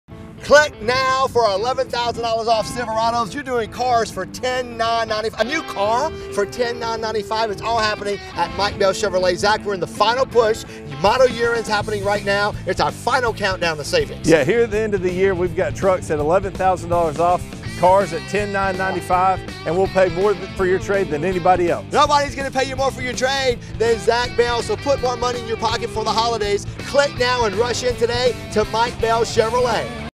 Black Friday Savings at Mike Bell Chevrolet
Save big this November on a new Chevrolet truck, sedan or SUV. We have more than 200 new Chevrolet models to pick from!